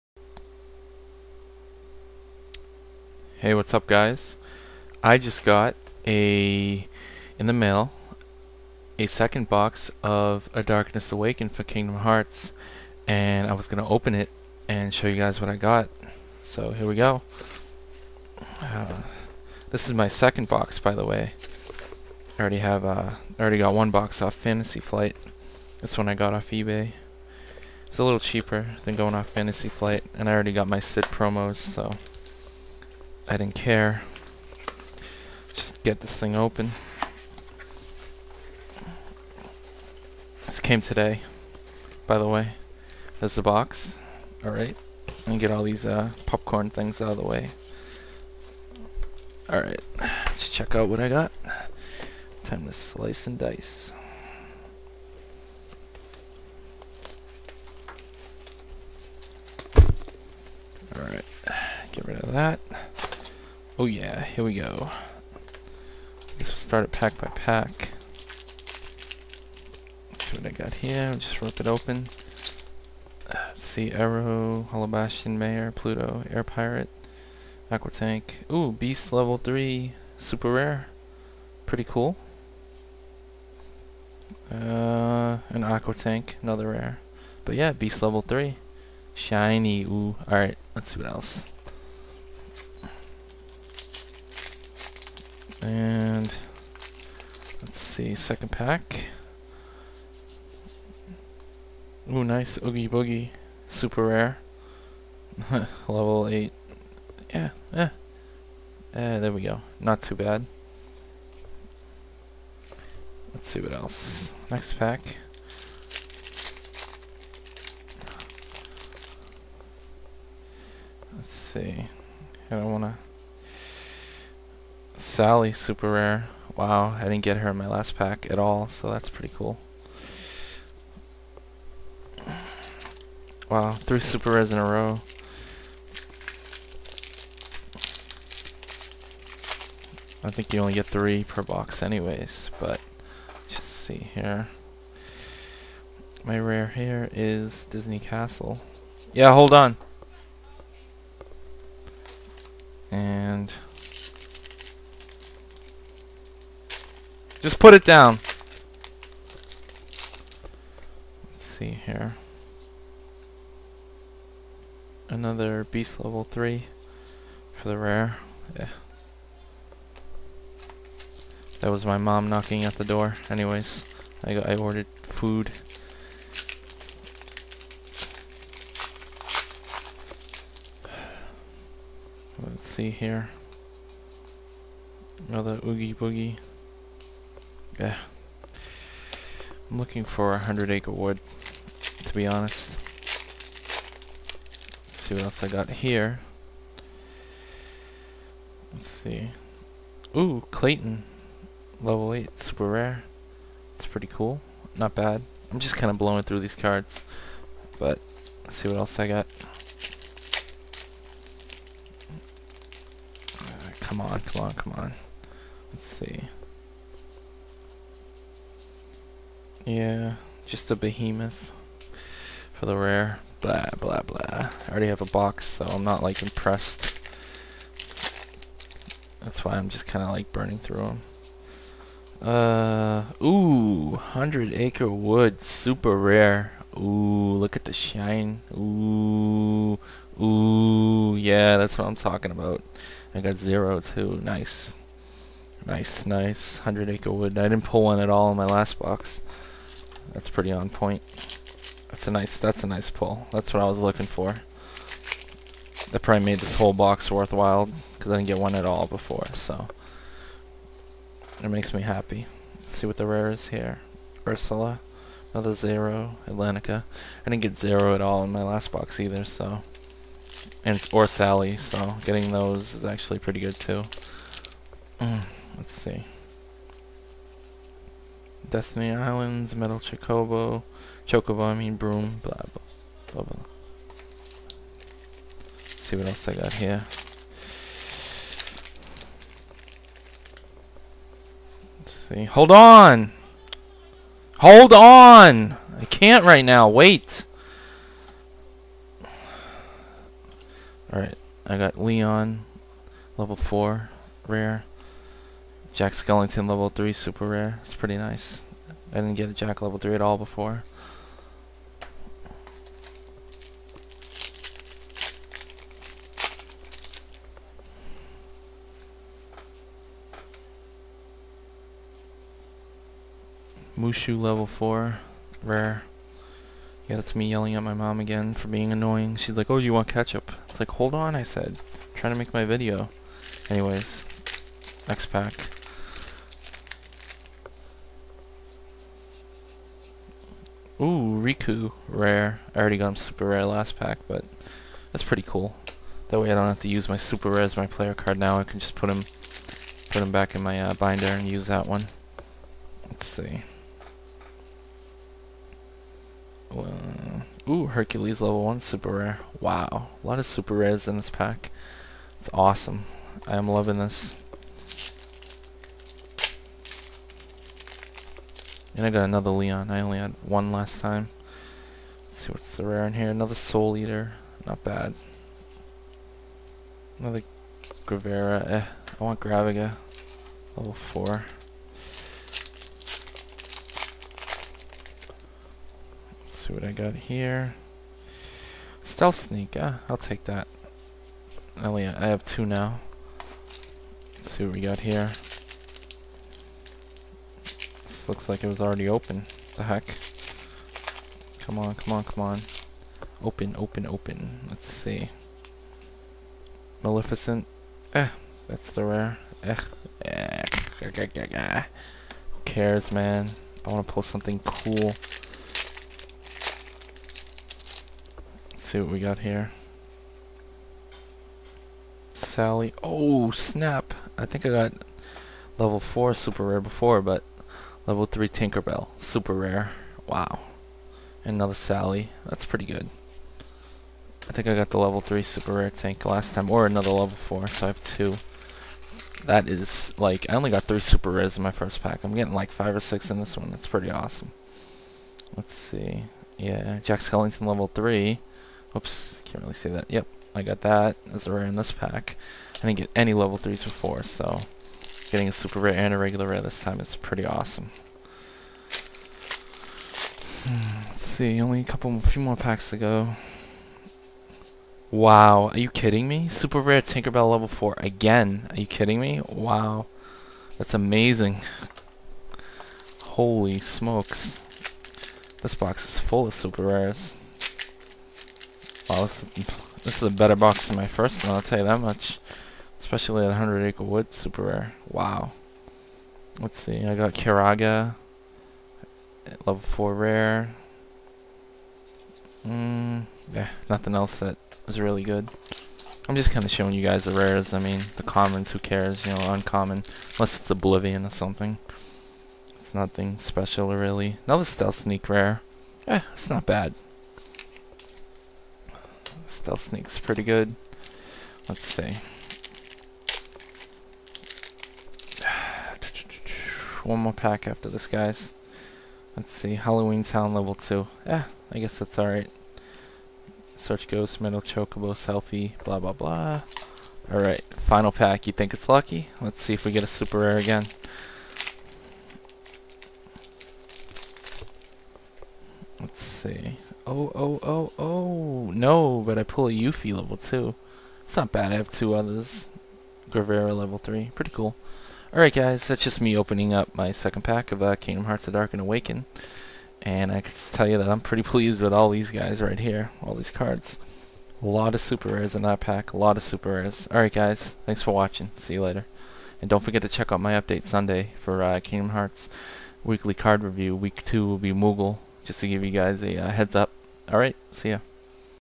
Opening Kingdom Hearts Packs from A Darkness Awakened
This is me opening up my second box of Kingdom Hearts A Darkness Awakened..

Super Rare Pulls:

Hercules Level 1
Tinkerbell Level 3
Tinkerbell Level 4
Beast Level 3
Oogie Boogie Level 8
Sally Level 0
Hundred Acre Wood Level 0
Clayton Level 8
Jack Skellington Level 3